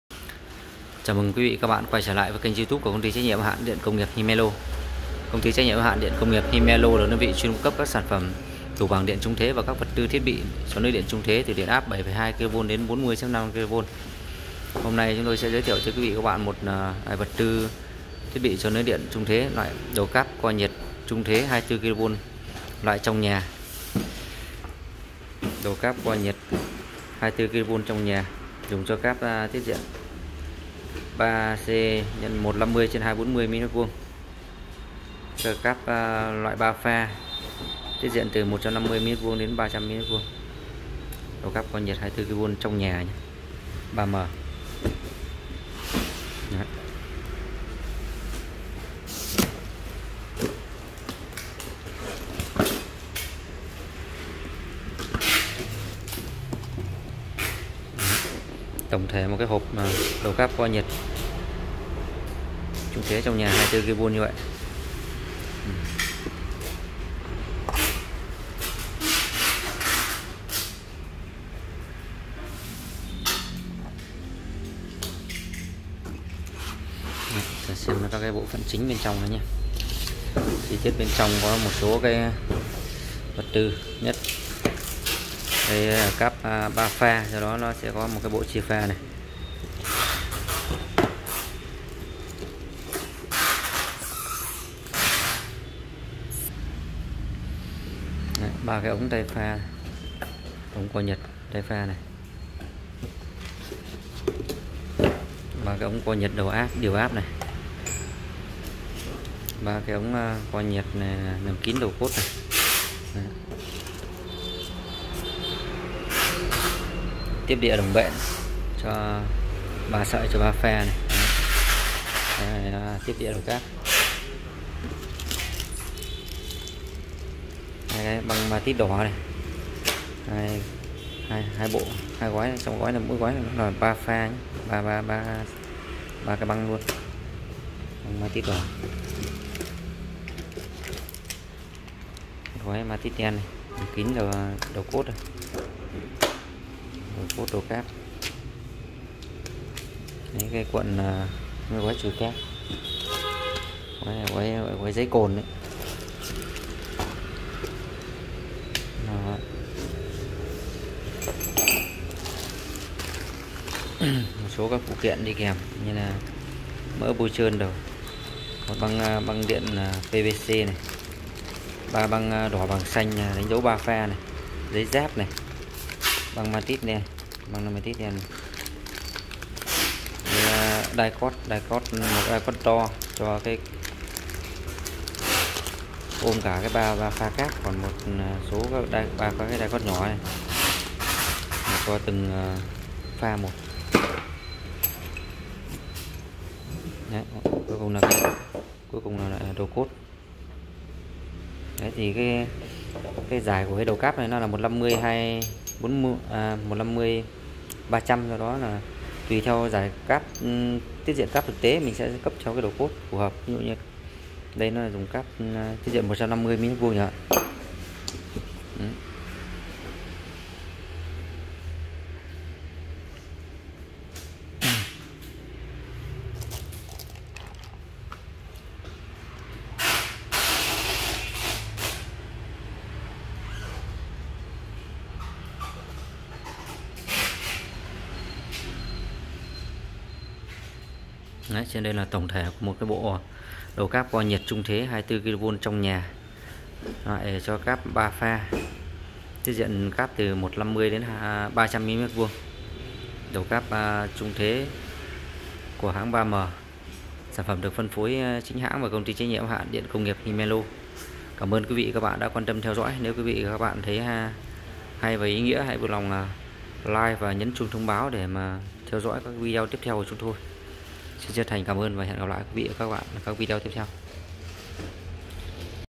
Review đầu cáp co nhiệt 24kV trong nhà 3M 3Cx150/300mm2
Giới thiệu đầu cáp co nhiệt 24kV trong nhà 3M 3Cx150/300mm2 - Đầu cáp co nóng loại trong nhà cho cáp 3 pha tiết diện 150mm2 đến 300mm2, sản phẩm đầu cáp ngầm trung thế 3M được phân phối chính hãng bởi Công ty TNHH điện công nghiệp Himelo.